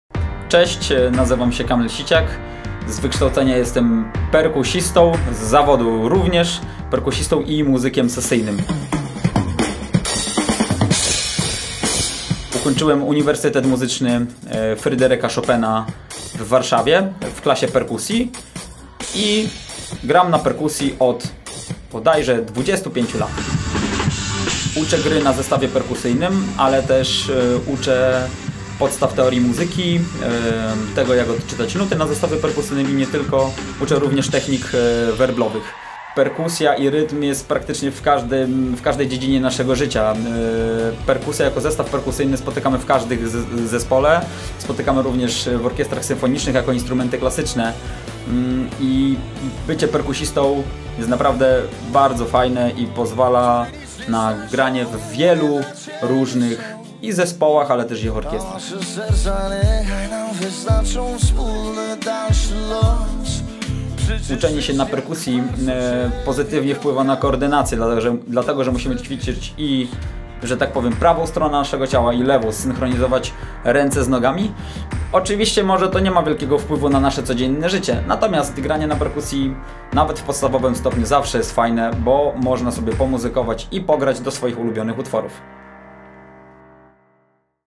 Kamil Siciak o perkusji!
Kolejna odsłona sylwetek nauczycieli Szkoły Muzyki z Pasją - Music and You w Markach!

Urodzony w Rzeszowie perkusista i muzyk sesyjny.
Absolwent warszawskiego Uniwersytetu Muzycznego Fryderyka Chopina w klasie
perkusji. Etatowy bębniarz w zespołach takich jak: Halina Młynkowa, Kamil Bednarek (w ramach Bednarek Inaczej), Rzeszów Klezmer Band, Klezmafour. Współpracował z wokalistami Bovska, Kayah, Edyta Geppert, The Dumplings, Owca, Jan Smoczyński, Urszula Dudziak, Dagadana, Betlejem w Polsce oraz wielu wielu innych. Na jego artystyczny życiorys składają się liczne występy, niemal na całym świecie z czołowymi artystami polskiej sceny muzycznej, orkiestrami symfonicznymi, udział w prestiżowych festiwalach oraz projektach artystycznych związanych z muzyką współczesną, rozrywkową i etniczną. Gorący entuzjasta motocykli, brudnych brzmień i bębnów vintage.